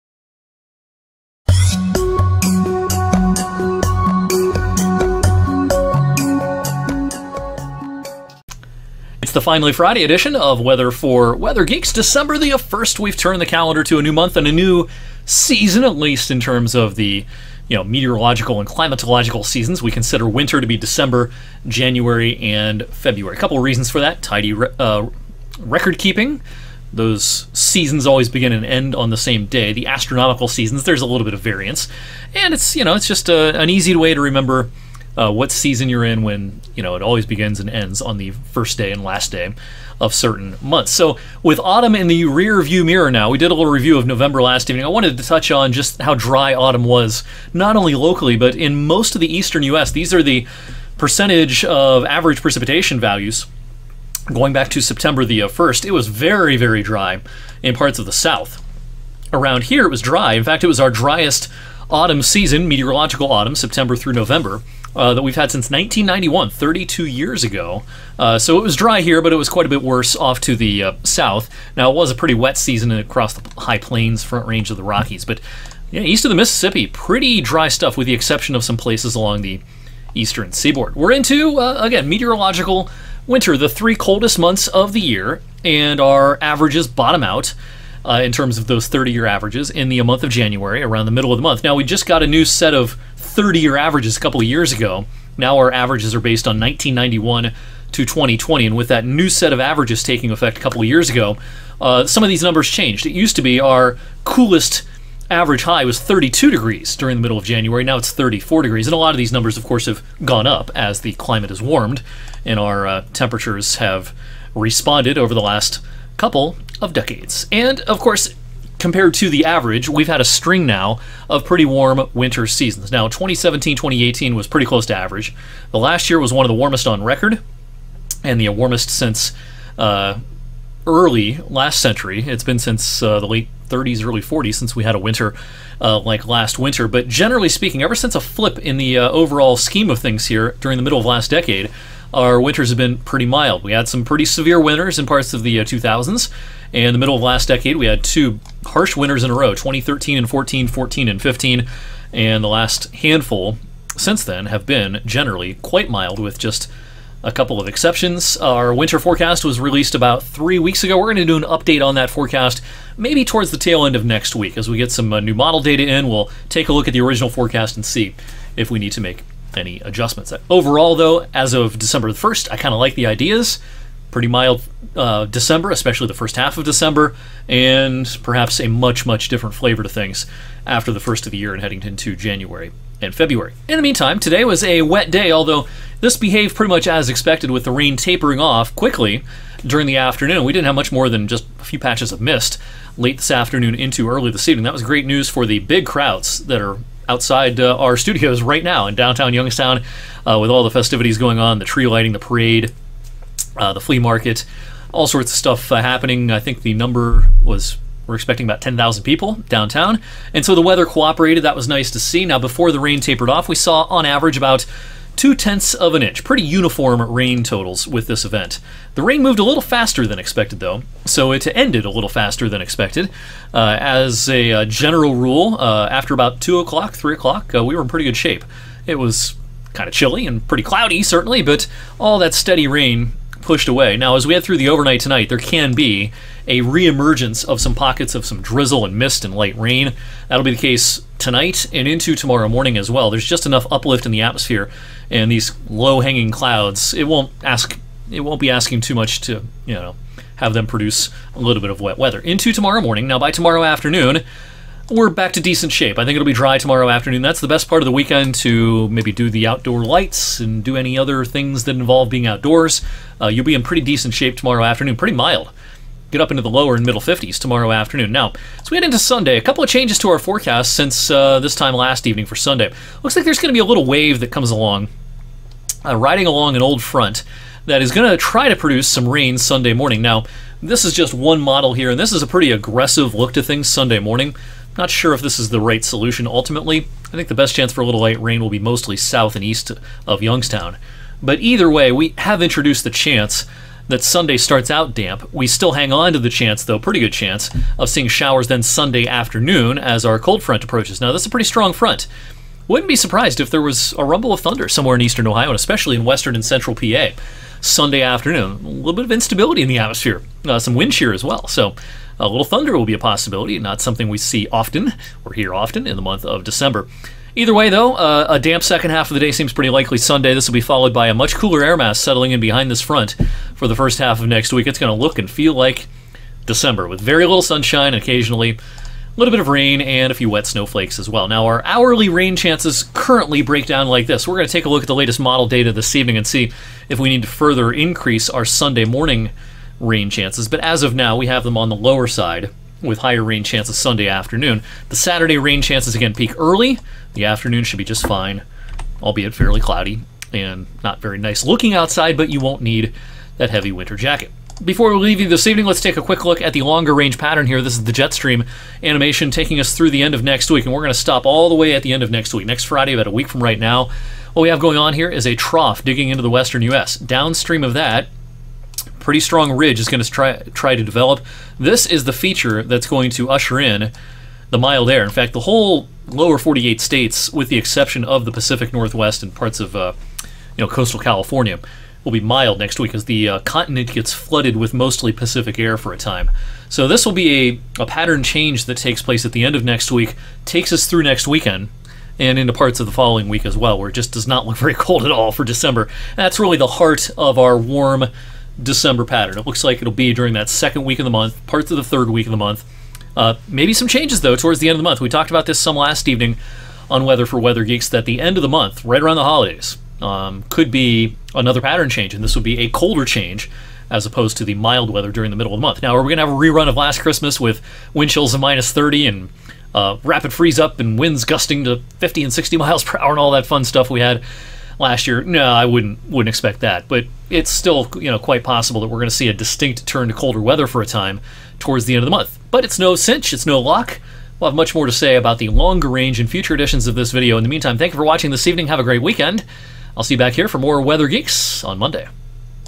Changeable December Weather; "Weather For Weather Geeks" 12/1/23
00:00 Intro
00:42 A Dry Autumn
01:18 Meteorological Winter Is Here
02:42 Today's Rain, Weekend Forecast
05:53 Jet Stream Next 10 Days